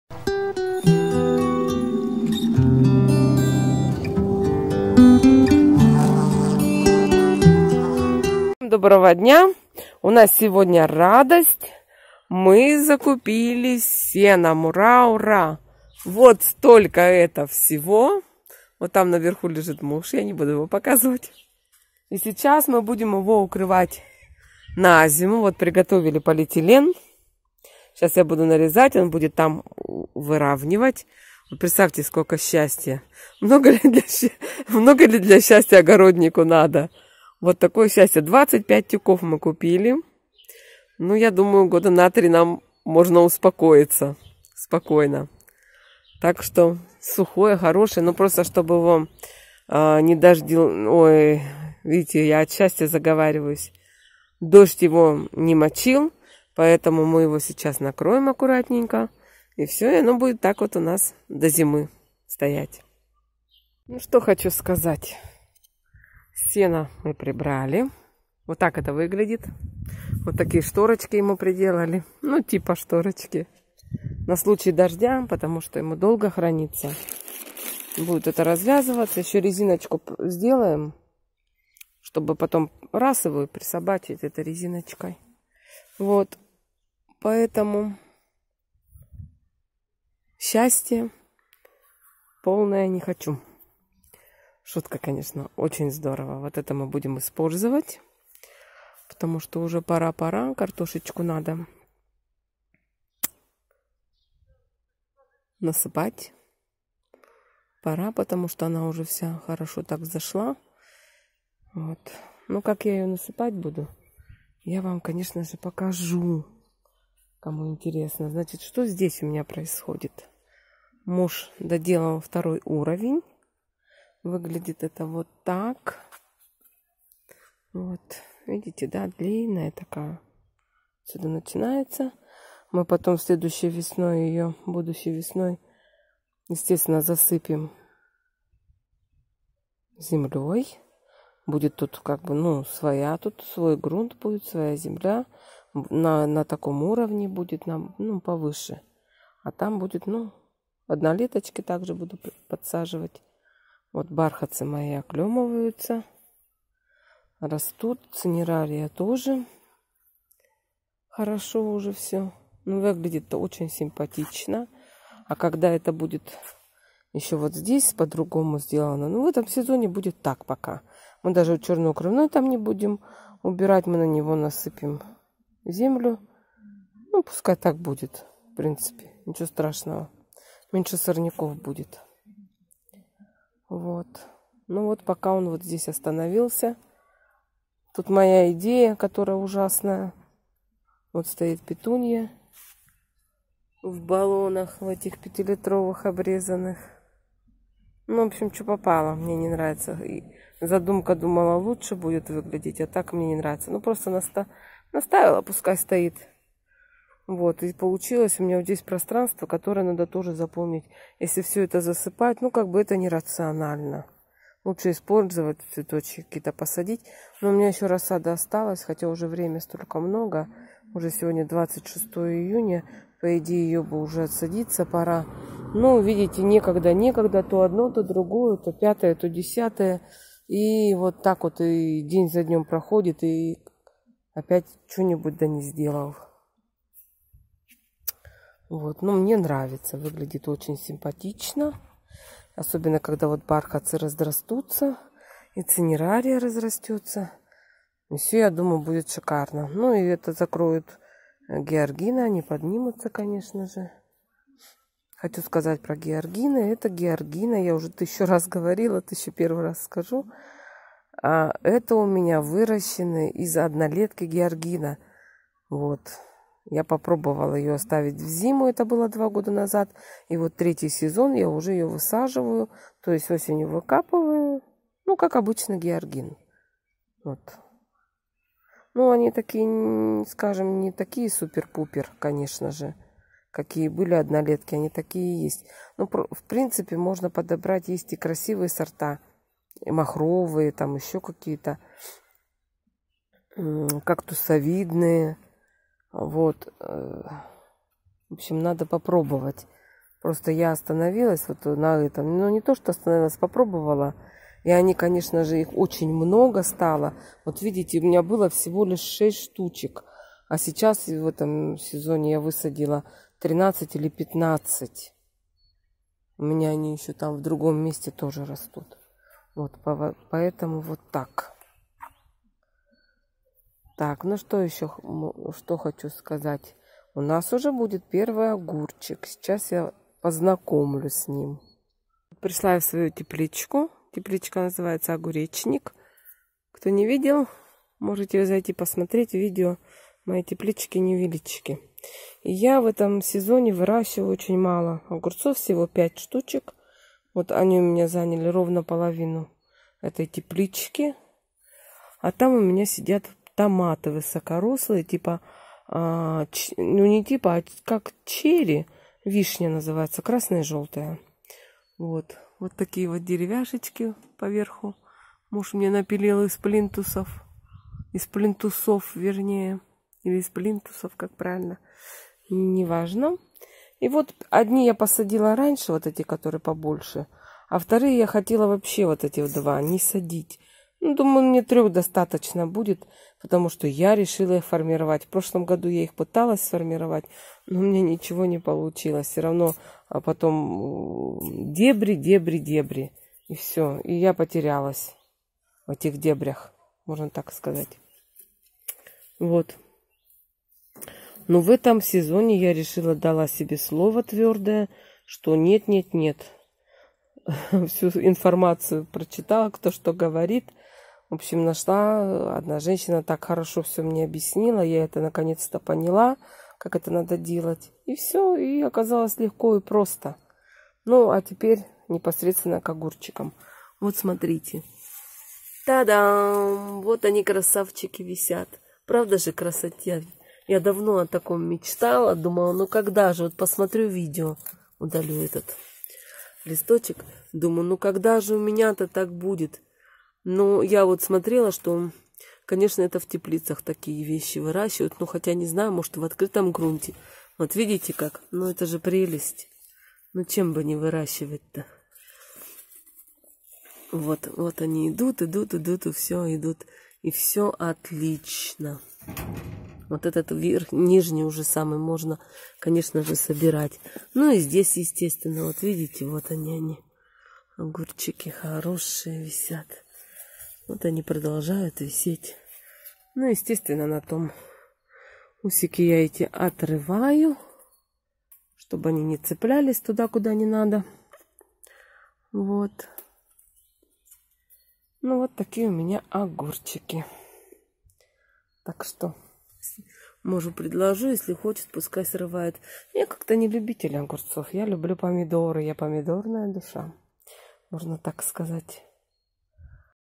СЧАСТЬЕ ОГОРОДНИКА//ОГУРЧИКИ//НАШ СЕЛЬСКИЙ УГОЛОК...
Приветствую заглянувших в гости на мой канал!!! Закупились!!! Счастье привалило))). Много ли надо огороднику... Огурчики пошли... и не много болталки...